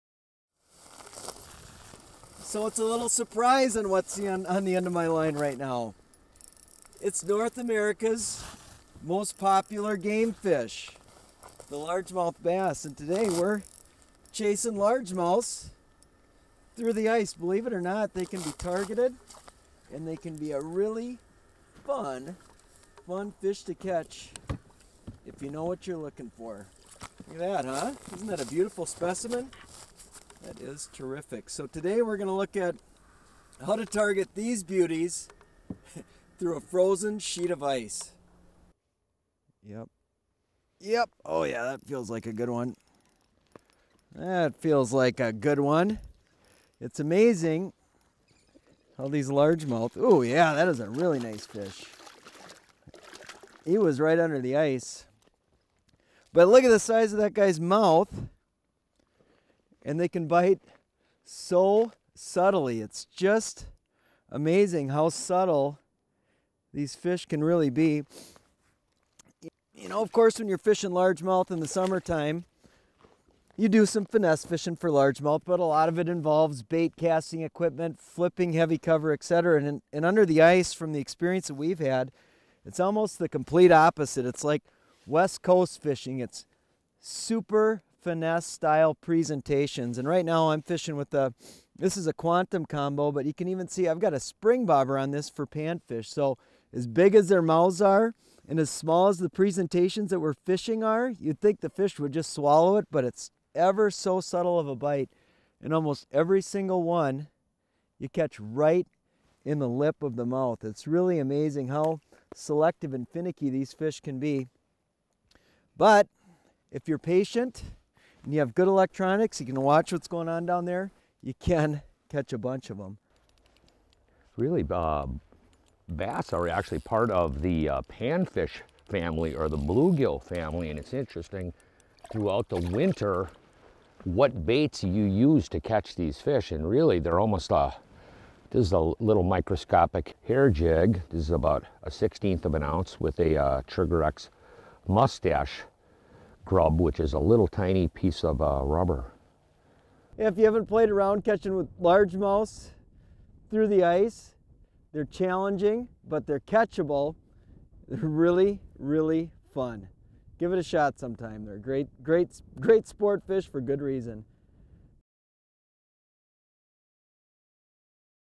Largemouth Bass Through the Ice
Not often targeted by us here at the edge during winter but Largemouth are still fun to look for in certain parts of the country where it's allowed. Join Jeremy Smith and James Lindner as they talk quickly about their latest tactics for pulling up winter Largemouth.